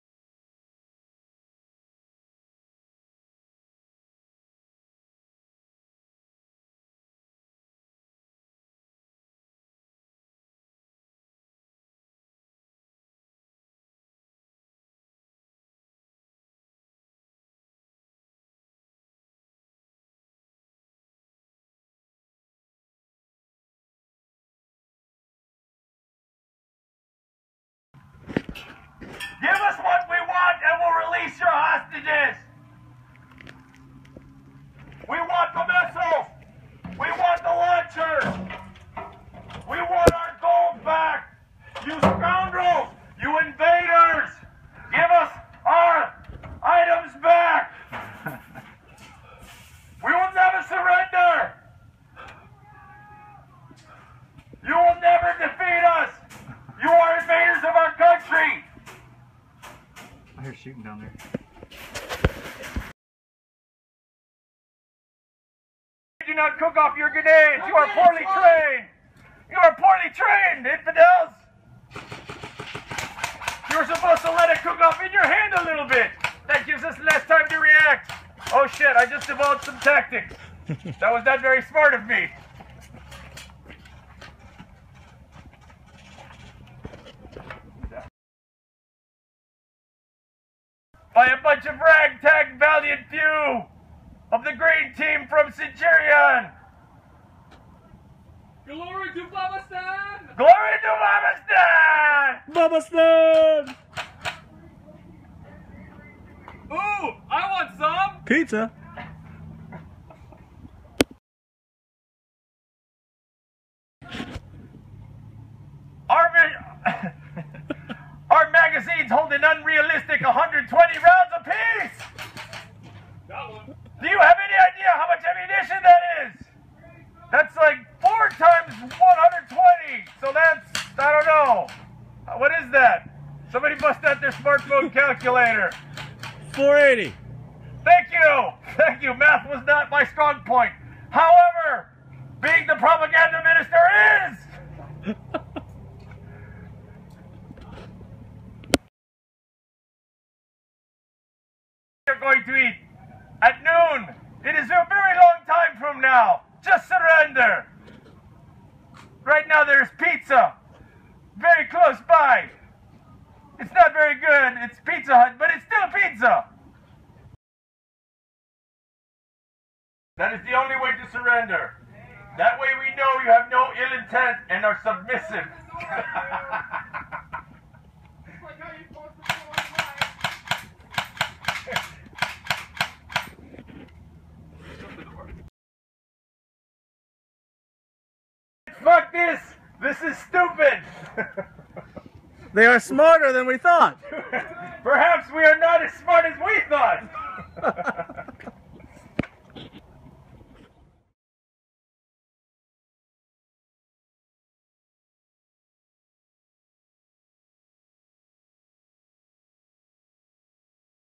Operation Stonewall: How to Make Enemy Troops Surrender
At Operation Stonewall some disruptive troops along with the 'Propaganda Minister' took a roof and began harassing the enemy... here are some great examples of how to give the enemy every opportunity to surrender while mostly making fun of them at the same time.

Thanks to all the support staff of Centurion and MSATO for making an enjoyable OPERATION in such a short time - although there were some issues, the players seem to have a good time and the weather ended up being fantastic.